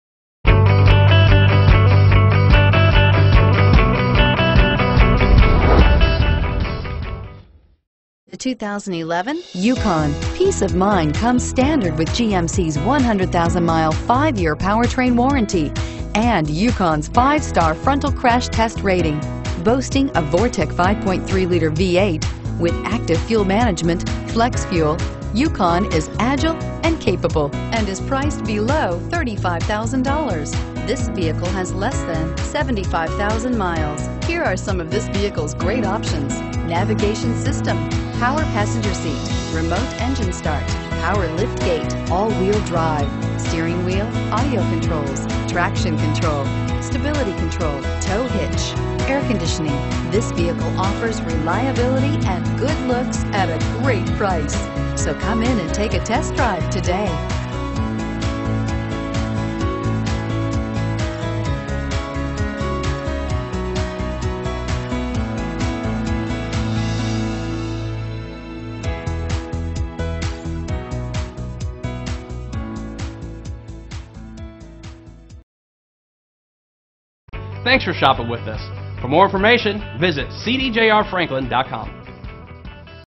2011 GMC Yukon Franklin TN L563103A
2011 GMC Yukon 1500 Denali AWD

Pick up this White Diamond 2011 GMC Yukon, available today at Chrysler Dodge Jeep Ram of Franklin. Featuring Automatic transmission, it has 73875 miles. This could be the one you've been searching for.